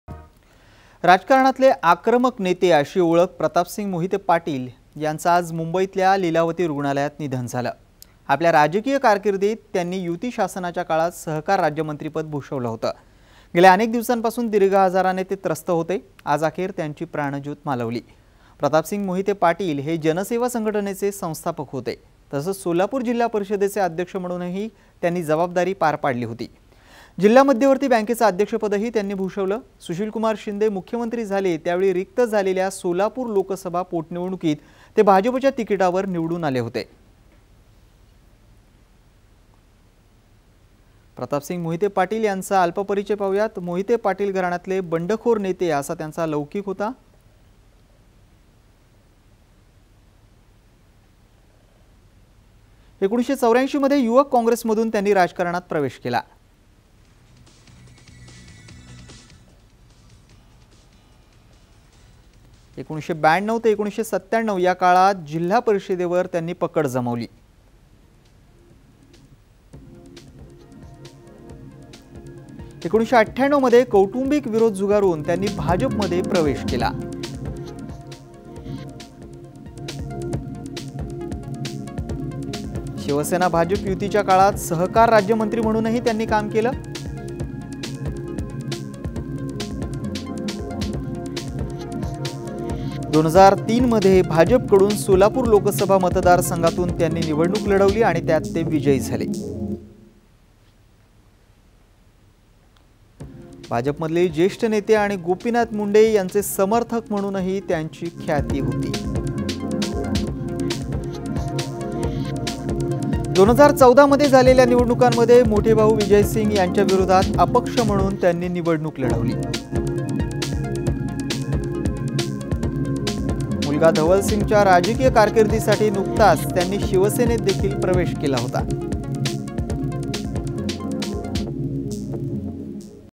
Former Maharashtra Minister Pratapsinh Mohite Patil passes away
Former Maharashtra Minister Pratapsinh Mohite-Patil passed away at Lilavati hospital after a brief illness. Pratapsinh, brother of former Deputy Chief Minister Vijaysinh Mohite Patil, was about 60 years old. He was the Minister for Cooperation in the Shiv Sena-led state government in the mid-90s.